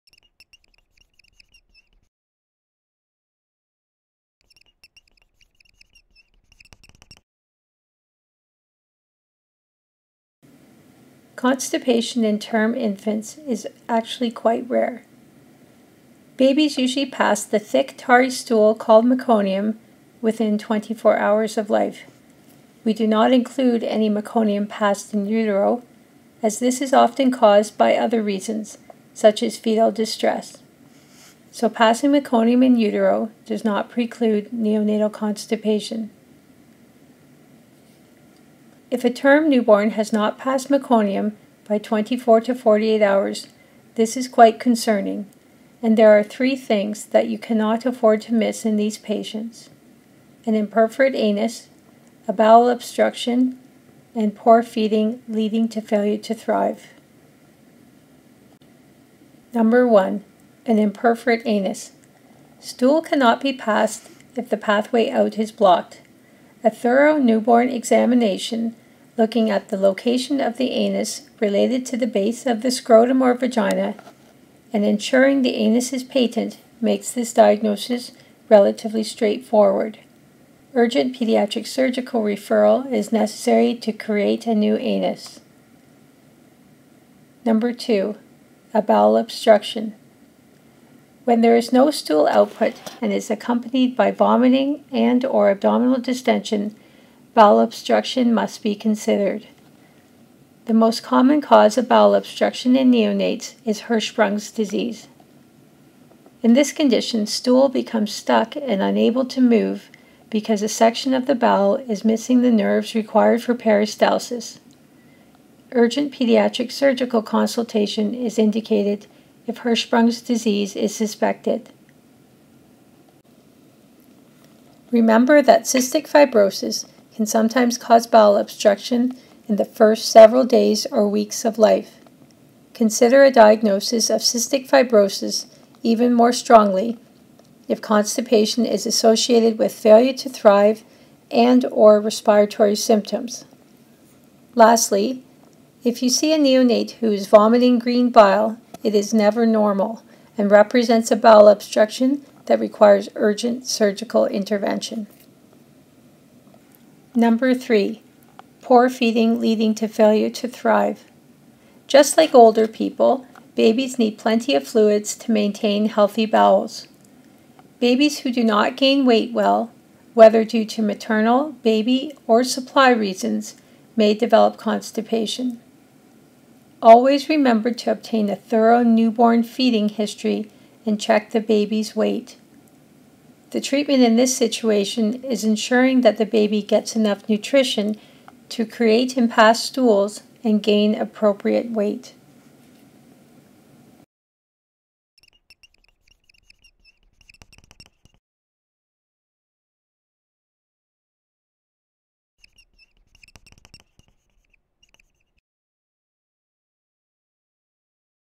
Pediatrics – Neonatal Constipation: By Kristen Hallett M.D.
Pediatrics – Neonatal Constipation
Whiteboard Animation Transcript
with Kristen Hallett, MD

Constipation in term infants is actually quite rare.

Babies usually pass the thick tarry stool called meconium within 24 hours of life. We do not include any meconium passed in utero, as this is often caused by other reasons, such as fetal distress. So passing meconium in utero does not preclude neonatal constipation.

If a term newborn has not passed meconium by 24-48 hours, this is quite concerning and there are three things that you cannot afford to miss in these patients – an imperforate anus, a bowel obstruction, and poor feeding leading to failure to thrive.

1. An Imperforate Anus

Stool cannot be passed if the pathway out is blocked. A thorough newborn examination looking at the location of the anus related to the base of the scrotum or vagina, and ensuring the anus is patent makes this diagnosis relatively straight forward.

Urgent pediatric surgical referral is necessary to create a new anus.

2. A Bowel Obstruction

When there is no stool output and it is accompanied by vomiting and/or abdominal distension, bowel obstruction must be considered.

The most common cause of bowel obstruction in neonates is Hirschsprung’s Disease. In this condition, stool becomes stuck and unable to move because a section of the bowel is missing the nerves required for peristalsis. Urgent pediatric surgical consultation is indicated if Hirschsprung’s Disease is suspected.

Remember that Cystic fibrosis can sometimes cause bowel obstruction in the first several days or weeks of life. Consider a diagnosis of cystic fibrosis even more strongly if constipation is associated with failure to thrive and/or respiratory symptoms.

Lastly, if you see a neonate who is vomiting green (bile), it is never normal and represents a bowel obstruction that requires urgent surgical intervention.

3. Poor Feeding leading to Failure to Thrive

Just like older people, babies need plenty of fluids to maintain healthy bowels.

Babies who do not gain weight well, whether due to maternal, baby, or supply reasons, may develop constipation.

Always remember to obtain a thorough newborn feeding history and check the baby’s weight. The treatment in this situation is ensuring that the baby gets enough nutrition to create and pass stools and gain appropriate weight.